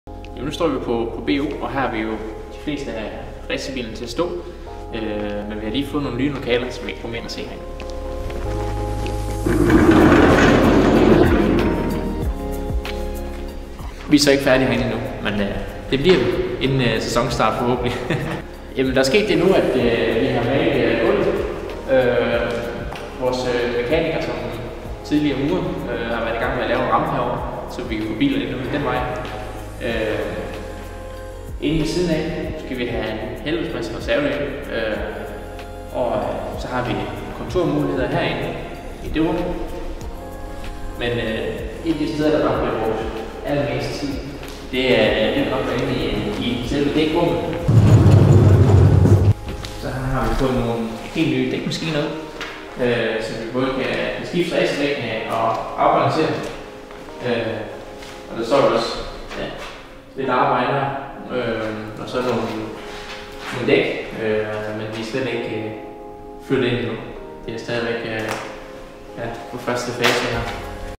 Tour at B&O with Mikkel O. Pedersen (in danish)
Mikkel is your tour guide and shows you around the new premises at B&O, where the cars are stored.